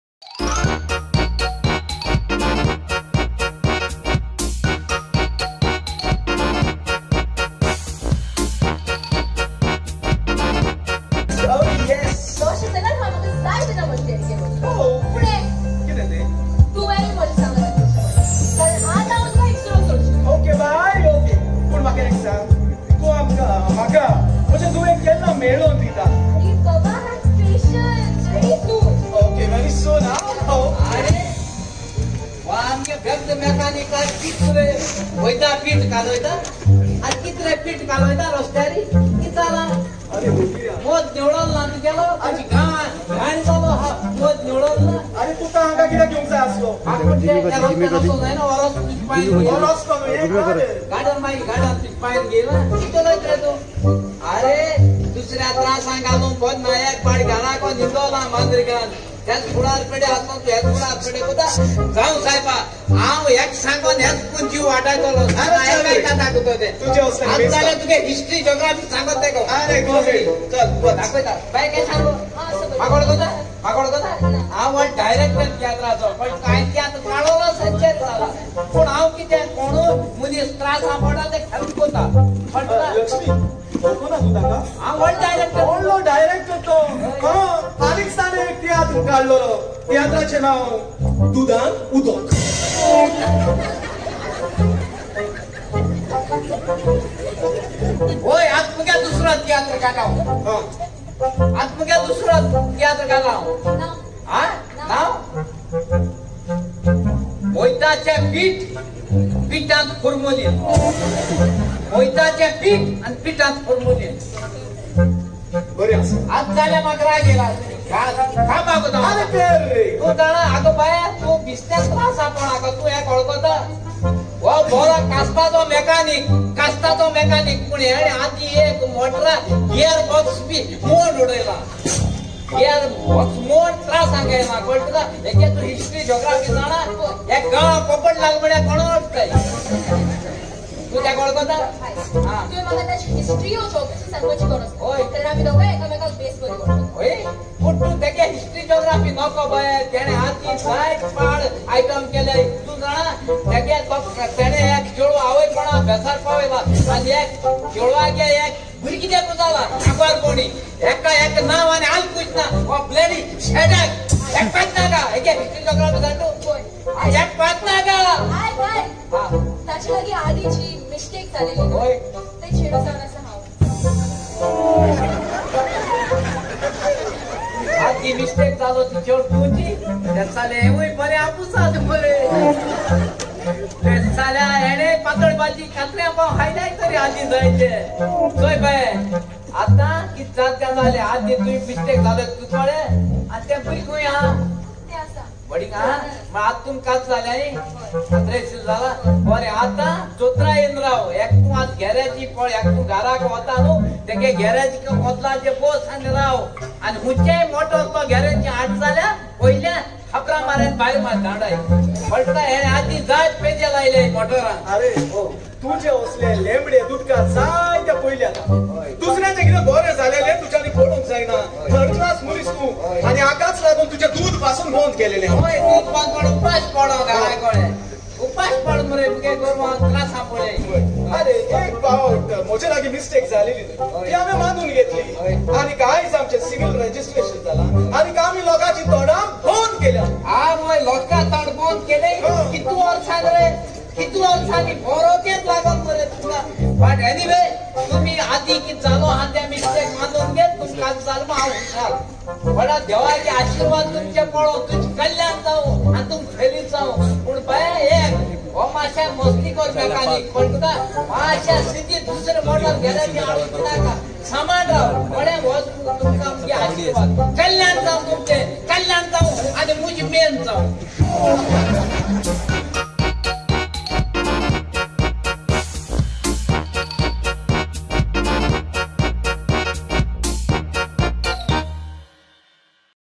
Konkani comedy 2020 by comedian Jesus antao
Stage comedy by natural comedian Jesus Antao along with Godwin Afonso and Shanaya Pereira.